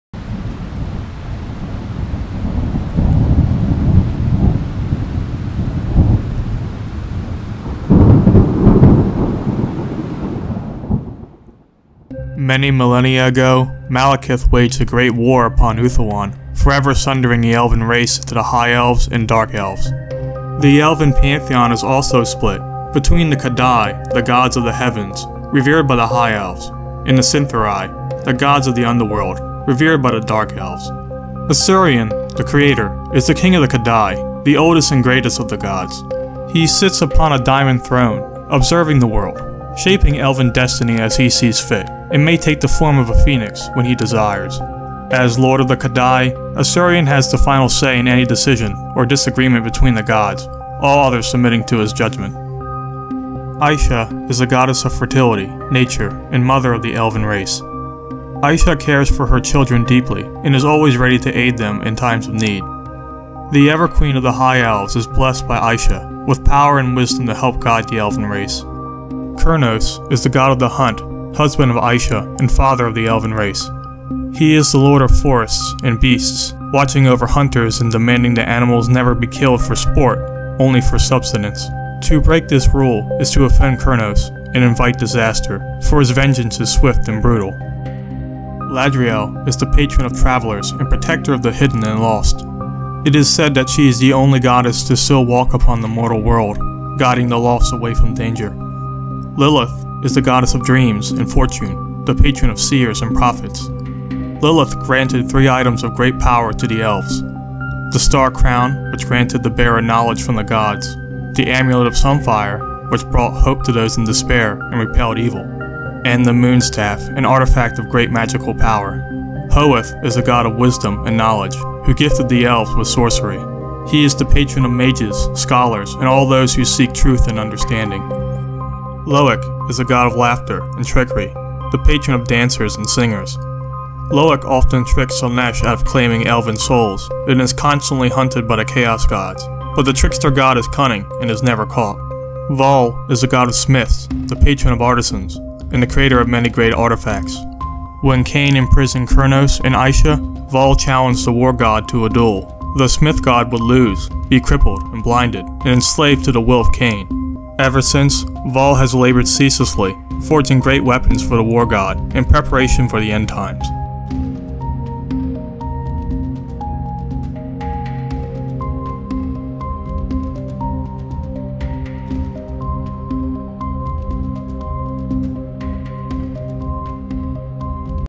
Rapid Fire - Warhammer Lord - Elven Pantheon - Part1 - The Cadai
In this video we take a quick look at the The Cadai, the gods of the heavens, of the Elven Pantheon